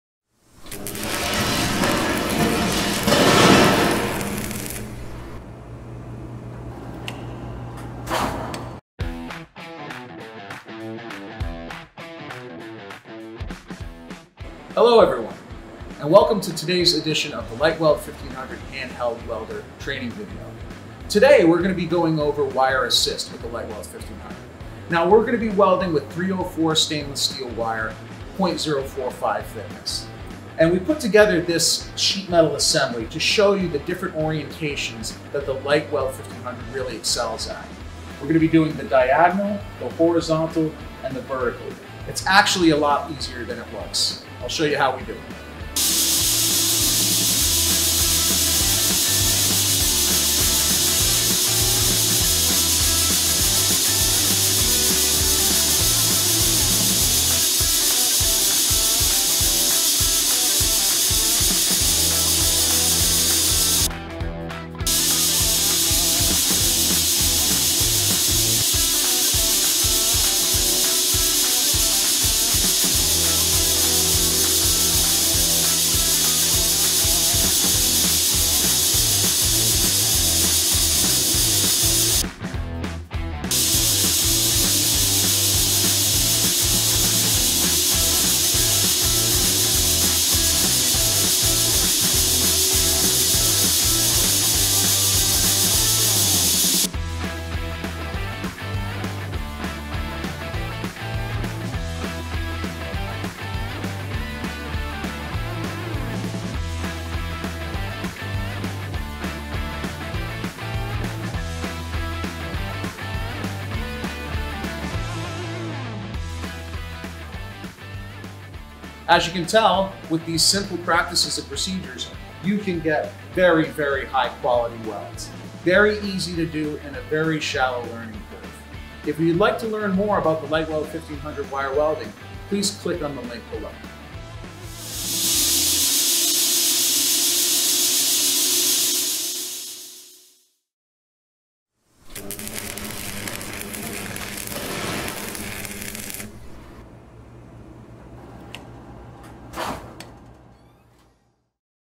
Laser Welding LightWELD 1500 with wire feed in 304 stainless 0 014 thickness
Fiber laser welding with wire assist with our LighWELD 1500 welding  304 stainless 0.045 inch material . Sold in Canada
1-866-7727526